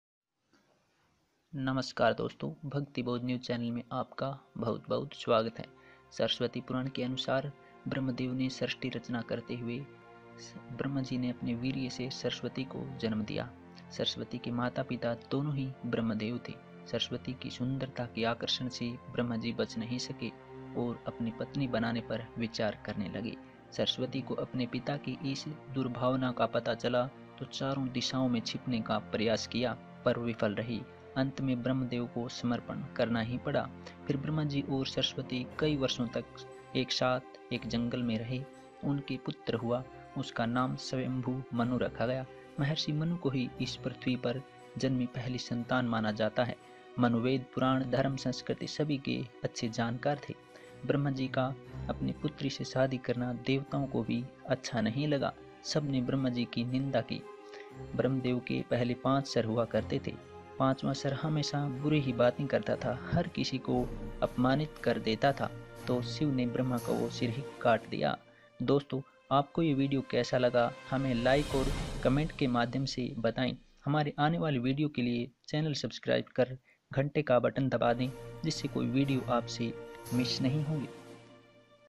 ब्रमदेव की सादी ये रिश्ता क्या कहलाता है What is the relation of Bramdev's Sadi
नमस्कार दोस्तों भक्ति बौद्ध न्यूज़ में आपका स्वागत है आज के इस वीडियो में ब्रमदेव की सादी के बारे में बताया है

What is the relation of Bramdev's Sadi

ब्रमदेव जब सृस्टि रचना कर रहे थे तब अपने वीर्य से सावित्री को प्रकट किया

उसके बाद उसकी सुंदरता को देखकर ब्रमदेव वो रिश्ता भूल गए और उससे सादी करने की सोचने लगे

सावित्री अपने पिता की इस मनसा को भाँप गईं और चारों दिसाओं में छुपने का प्रयाश किया

पर छुप नहीं सकि अंत में समर्पण करना ही पड़ा उसके बाद   ब्रमदेव और सावित्री के मिल्न से मनु का जन्म हुवा

मनु का जन्म इस पृथ्वी पर सबसे पहला माना जाता है

Ghante Ka Batan Dabayen Jisse Koi Video Aapse Mis Nahi Honge Ye Bilkul Free Hota Hai